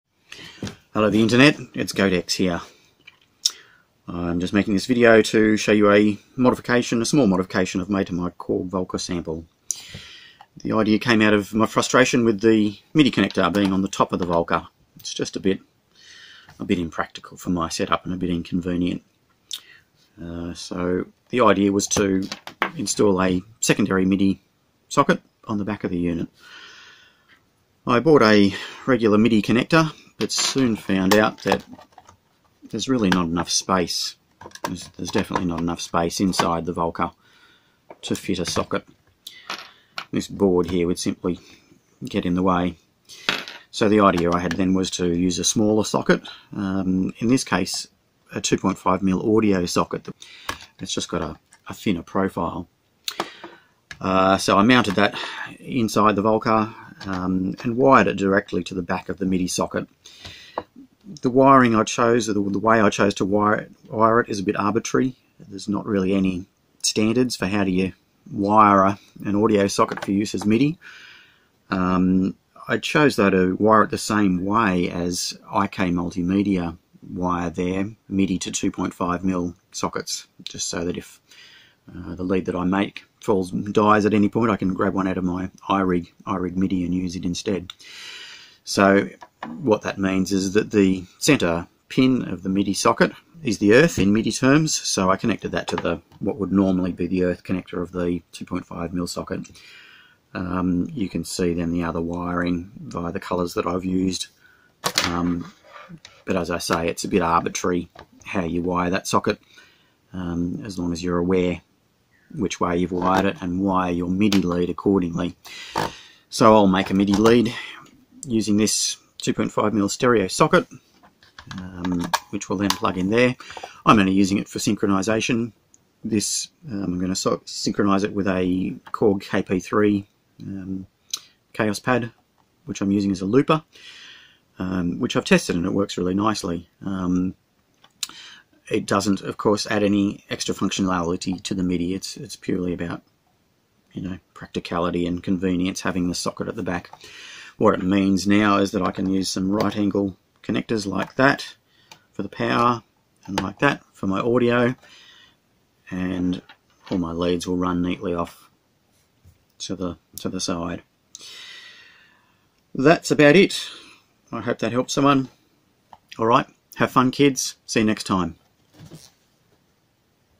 Volca Sample modification - second MIDI input
Here's a modification I've made to my Korg Volca Sample - adding another MIDI input.

Don't get excited - the mod doesn't add to the MIDI functionality of the VS, it just adds a secondary MIDI input to the back of the unit, for practicality and convenience (I found the top-mounted socket a PITA).

Note that you can only use one MIDI input at a time - this won't provide MIDI merging functionality.

Goat-X